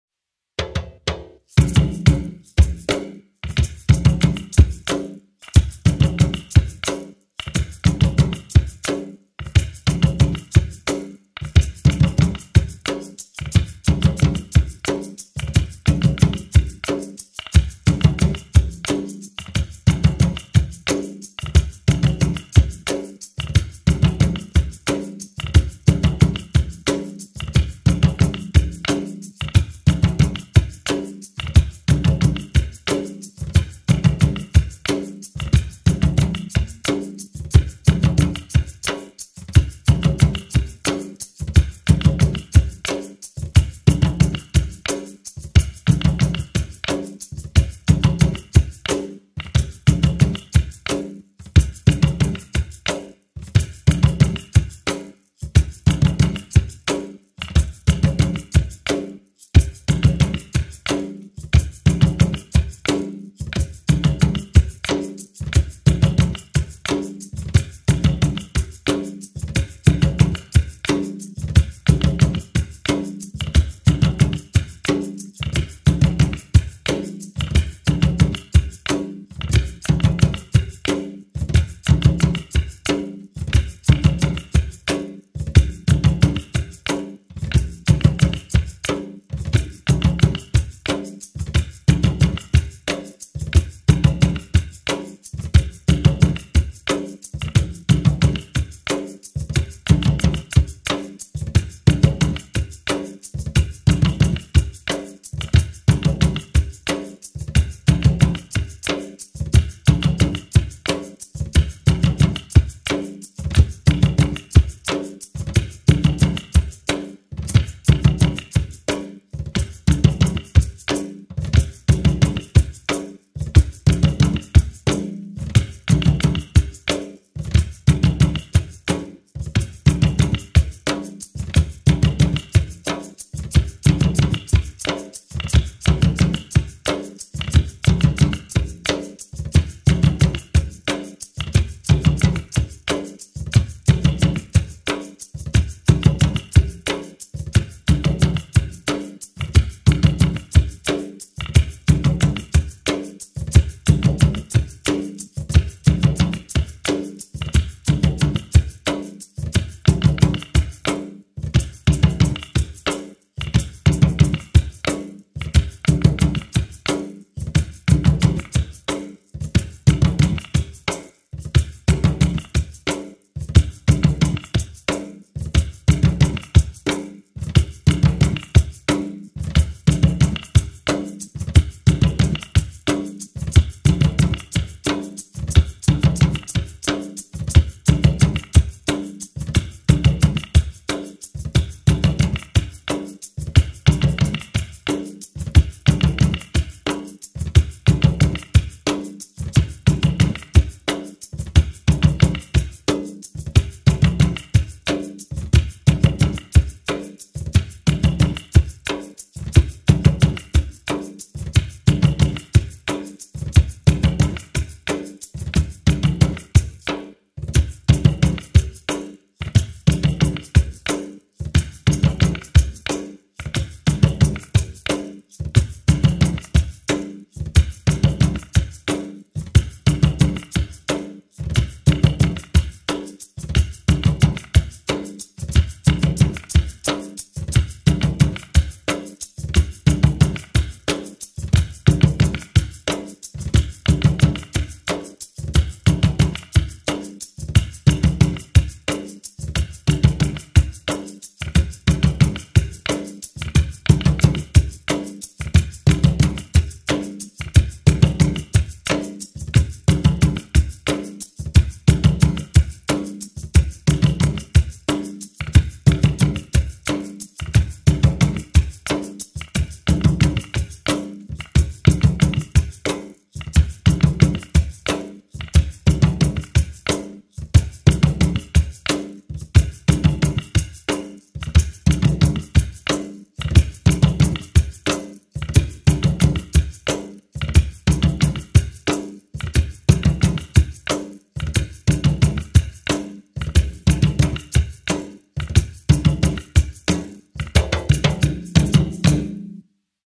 Bomba Puertorriqueña Ritmo Yubá Masón
Colección de ritmos de Bomba Puertorriqueña para la práctica de baile, canto, primo o buleo.

Puerto Rican Bomba rythms for dance, drumming and singing practice.